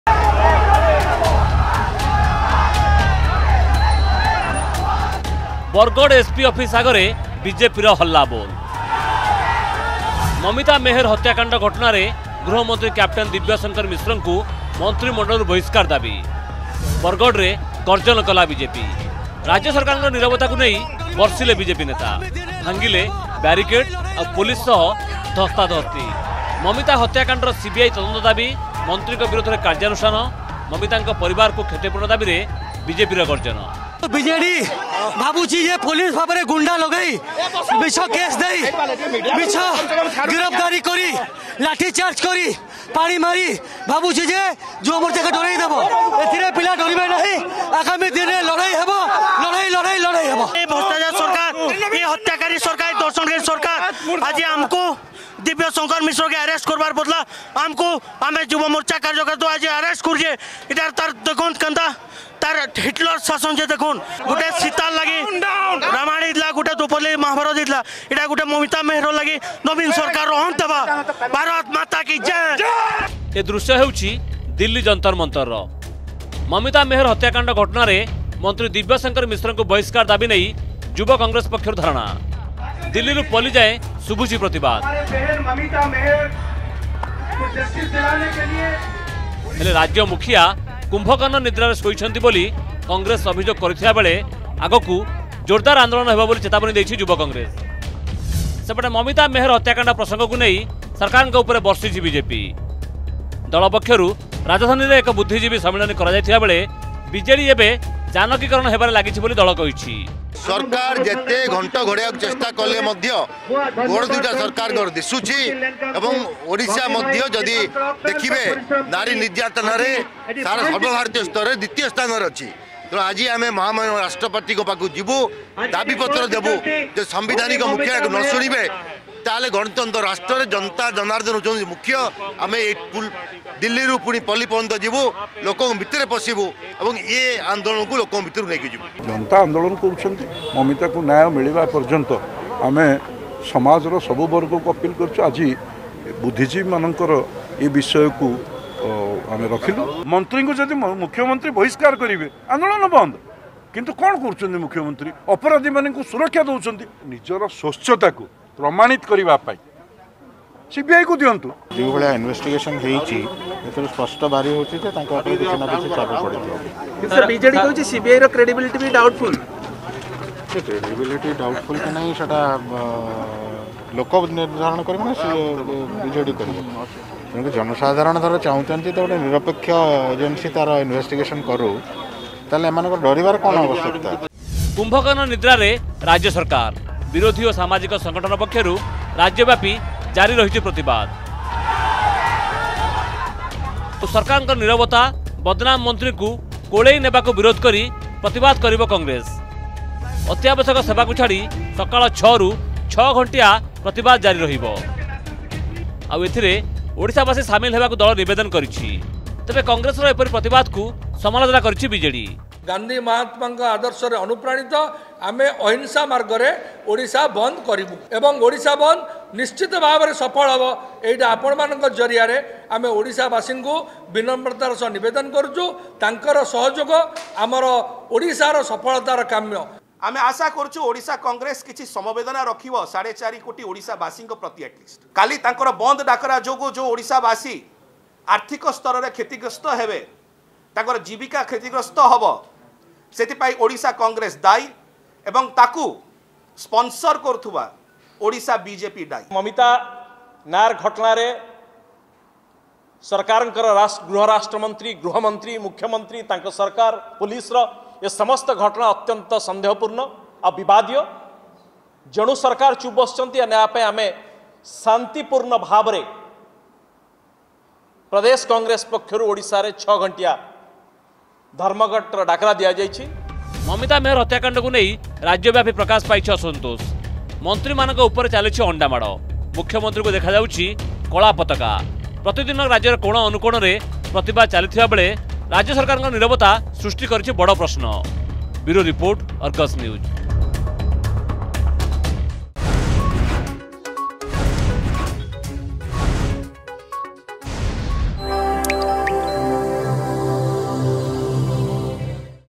Mamita Meher Case | ଥମୁନି ମମିତା ମେହେର ହତ୍ୟାକାଣ୍ଡ ତାତି
ବରଗଡ ଏସପି ଅଫିସ ଆଗରେ ବିଜେପିର ହାଲ୍ଲାବୋଲ୍ । ମମିତା ମେହେର ହତ୍ୟାକାଣ୍ଡ ଘଟଣାରେ ଗୃହମନ୍ତ୍ରୀ କ୍ୟାପଟେନ୍ ଦିବ୍ୟଶଙ୍କର ମିଶ୍ରଙ୍କୁ ମନ୍ତ୍ରୀମଣ୍ଡଳରୁ ବହିଷ୍କାର ଦାବି । ବରଗଡରେ ଗର୍ଜନ କଲା ବିଜେପି । ରାଜ୍ୟ ସରକାରଙ୍କ ନୀରବତାକୁ ନେଇ ବର୍ଷିଲେ ବିଜେପି ନେତା । ଭାଙ୍ଗିଲେ ବ୍ୟାରିକେଡ୍ ଆଉ ପୋଲିସ ସହ ଧସ୍ତାଧସ୍ତି ।